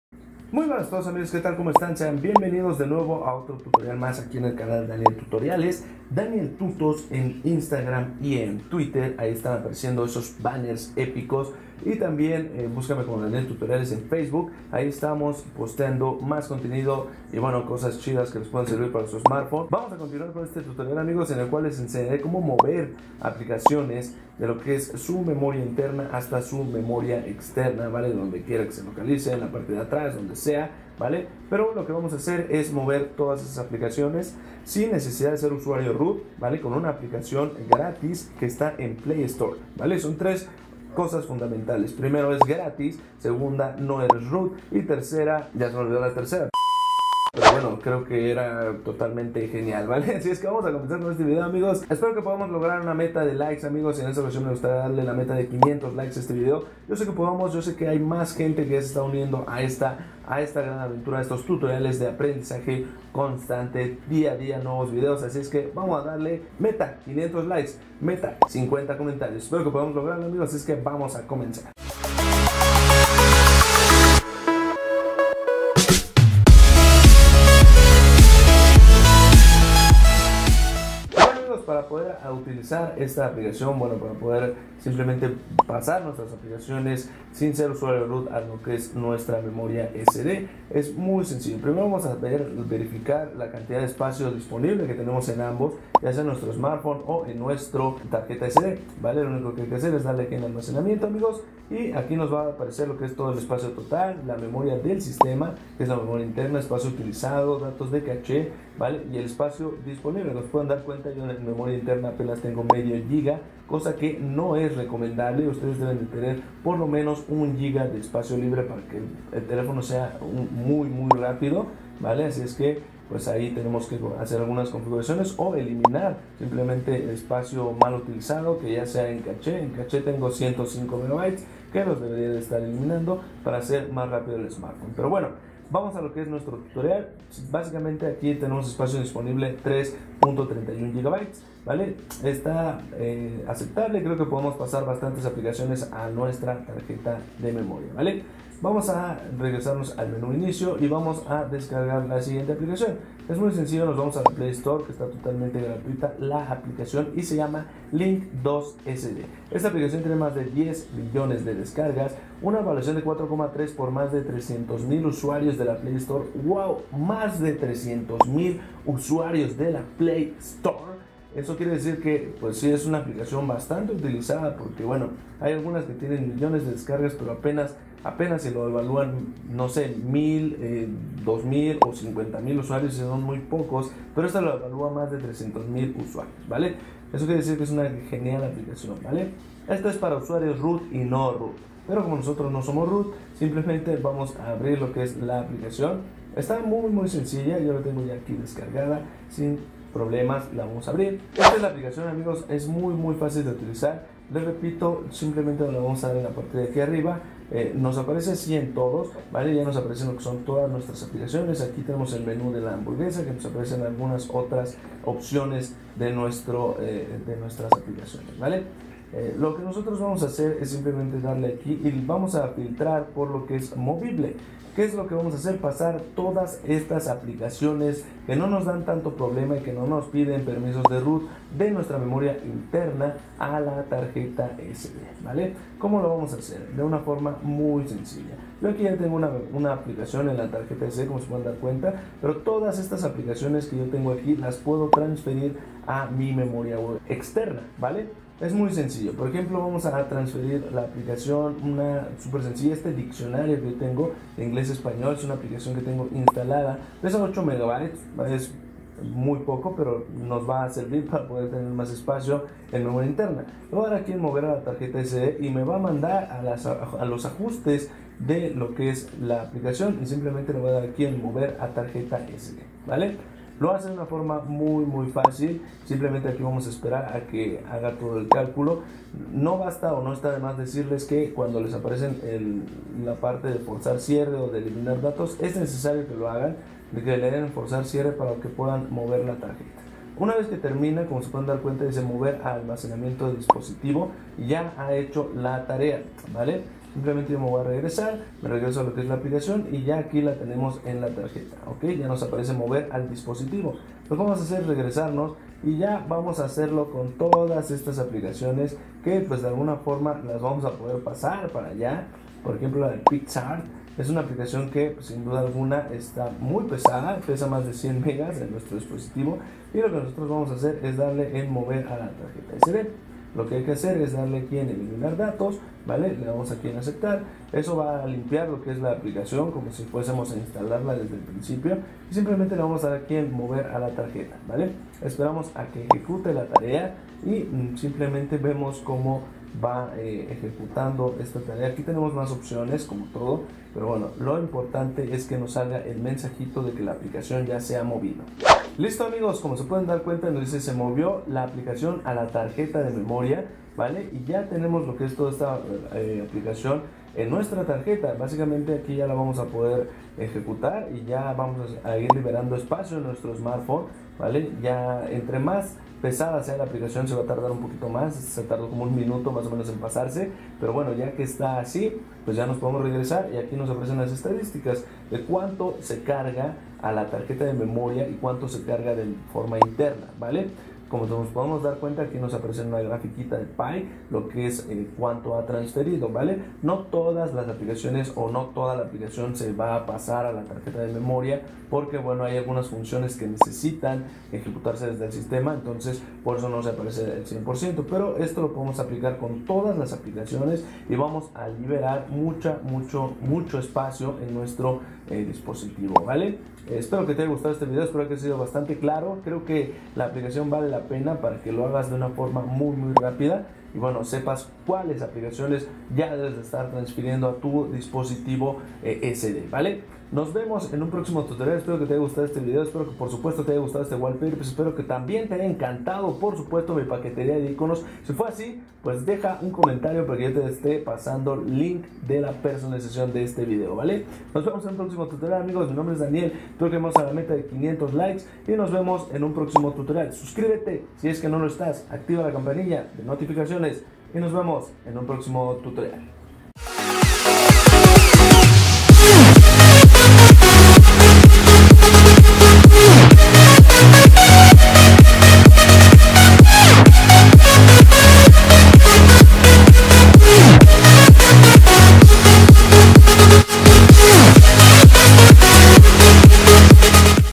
Como Mover Aplicaciones de Memoria Interna a Tarjeta SD / Sin Ser Root 2018
En este video te enseñare como mover aplicaciones de la memoria interna a la tarjeta SD de tu Android, te enseñare como mover las APPS e instalarlas en la memoria externa, Aprende que aplicaciones mover sin la necesidad de ser ROOT.

ORIGINAL.